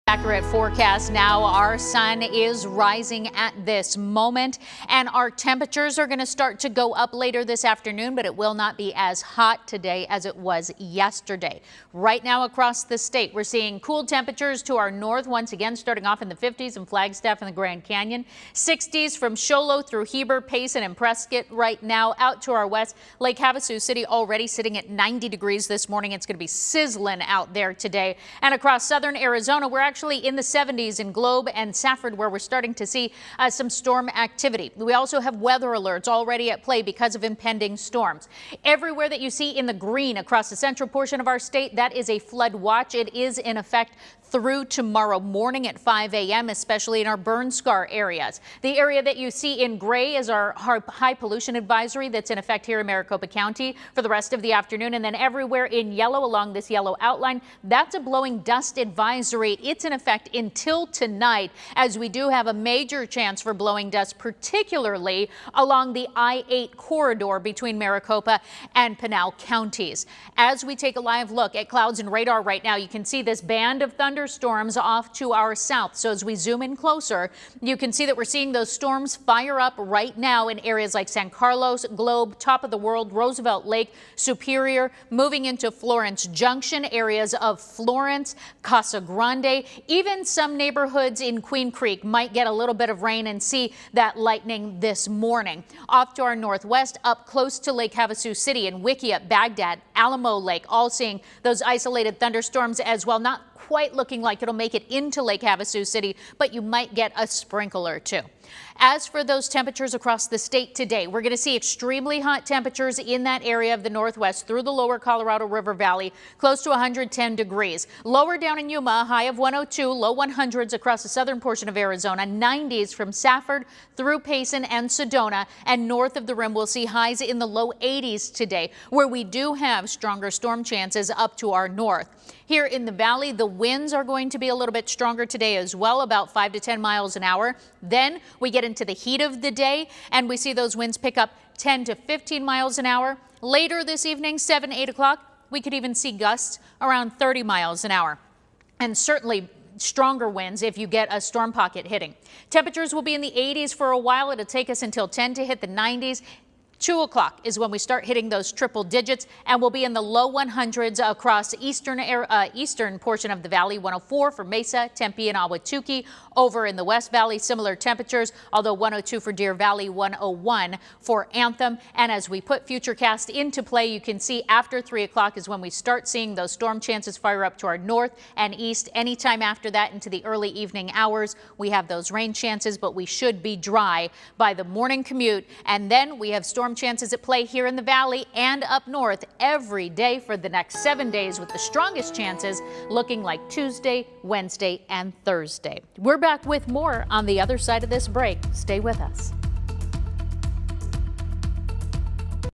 Monsoon rain chances ramping up
Monsoon rain chances ramp up again after a few dry days in the Valley.